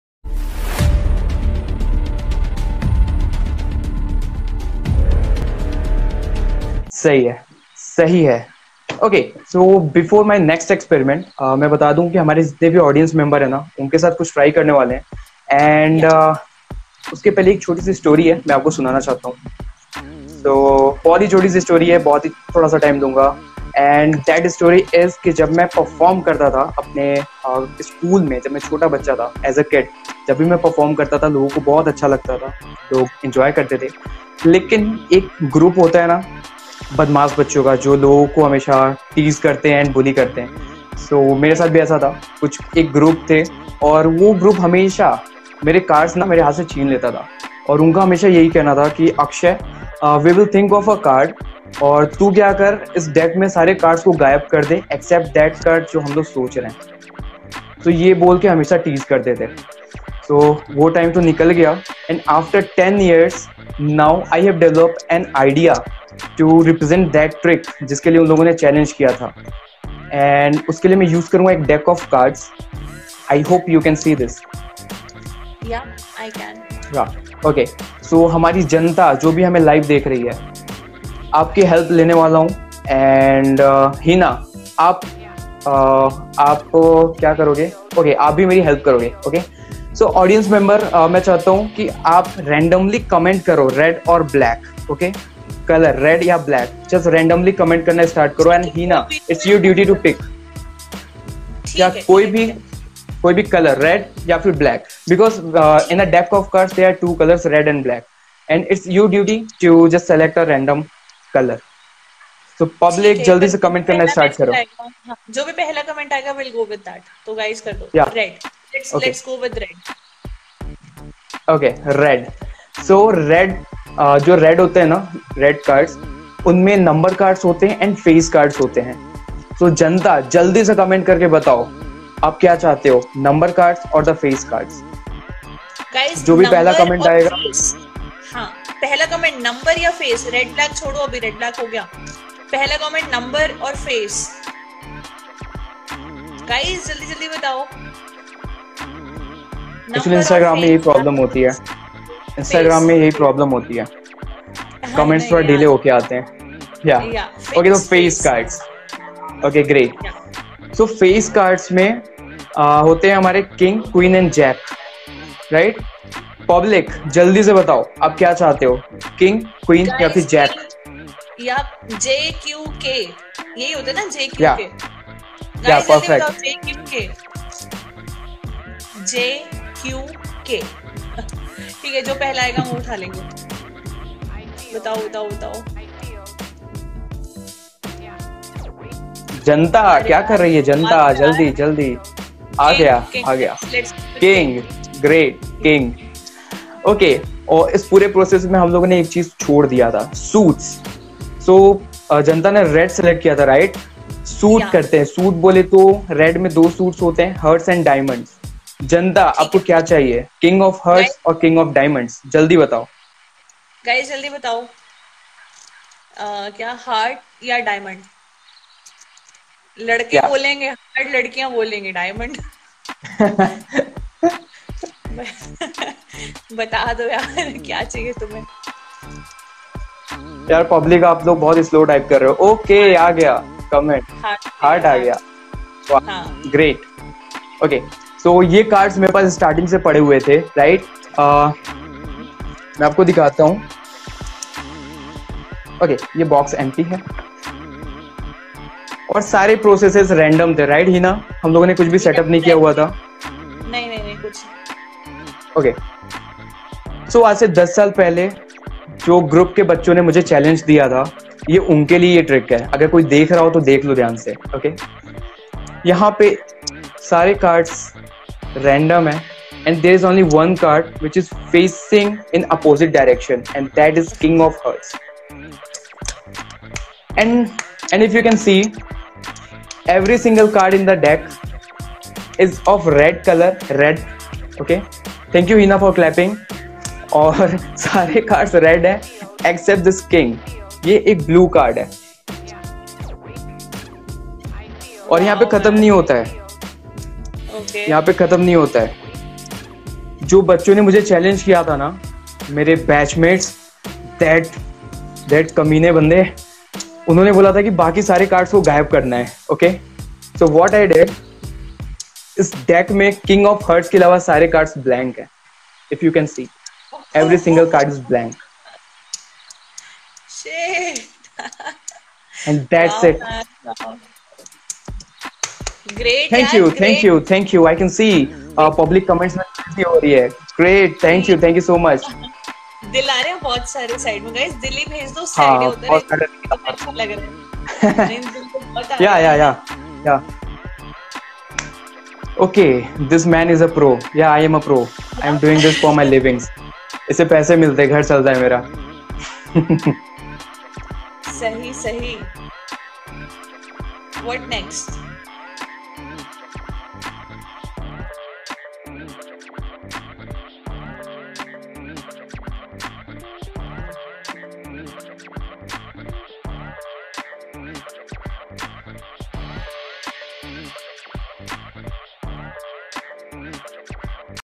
How to read Girls Mind 🔥|| Story telling and Mentalism in India || AKSHAY SINGH MAGIC - ASK
(show your support by liking the video)

Hey Public,
I hope you all are safe and doing great❤️ !!
in this video I did try something new and crazy.
(the story I shared in the video is only to make my effect look more genuine, in reality everyone was very supportive & I do love each of my friend❤️)

Do join me on social media to experience Live mind reading

live instagram live events girls reacts prank in lockdown